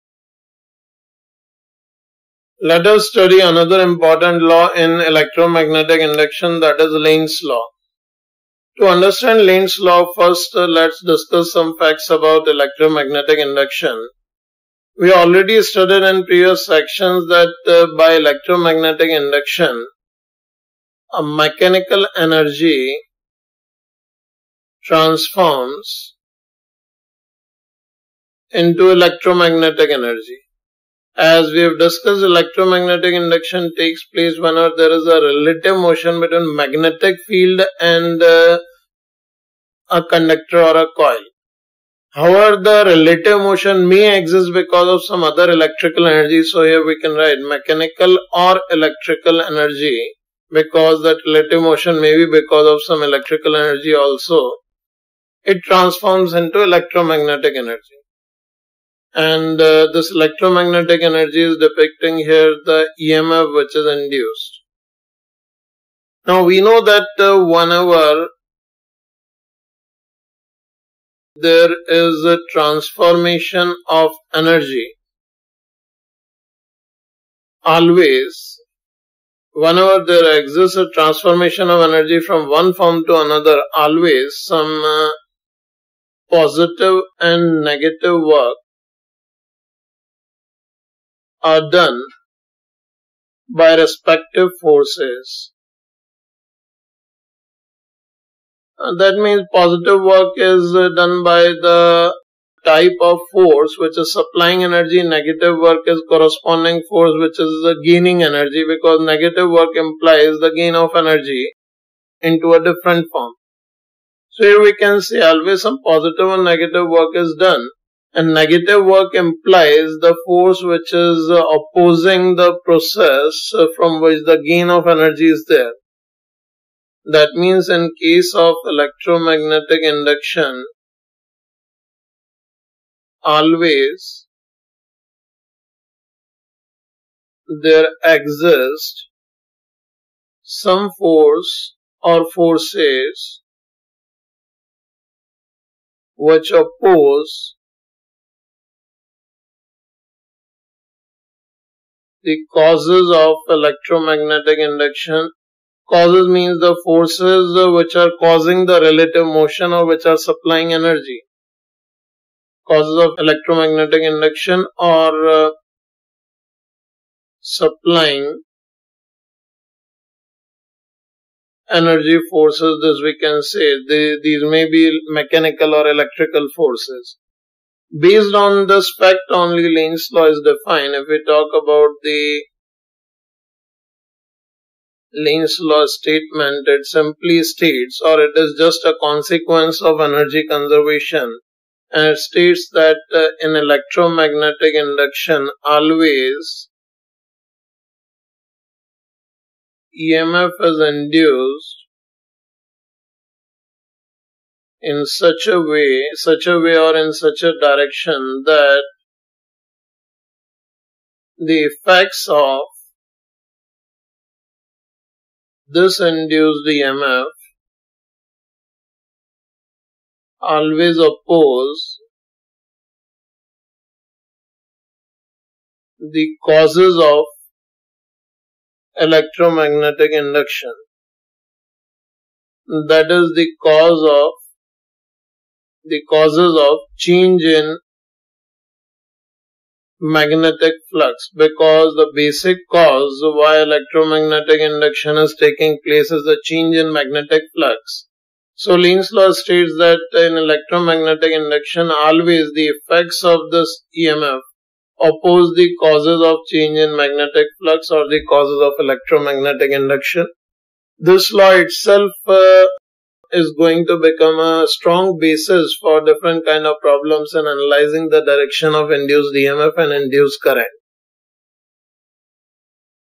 Class 12 Physics | Electromagnetic Induction | #4 Lenz's Law | For JEE & NEET
PG Concept Video | Electromagnetic Inductance | Lenz's Law by Ashish Arora

Students can watch all concept videos of class 12 Electromagnetic Inductance for jee & neet in proper sequence through PG channel playlist.